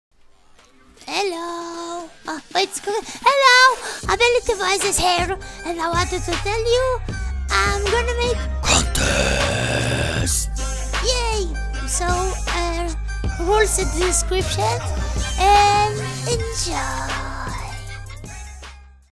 Covarrr Contest [READ DESC!!!!!]
Harro! :D
Rulessssss
1.Dont do it longer then 1 min :D
2.you can cover the all edit even yours :D
3.Dont change your voice using Voice Softwares
4.Comment the entry :D
Now :D Prizesss :D
1st Place - making Cover with me and He choosing the Edit/SubBox for 6 weeks
2nd Place - making Cover with me and im choosing the edit/SubBox for 4 Weeks
3rd Place - SubBox For 2 Weeks[srry For this idiot place]
Contest Closing Date - 14/06/2014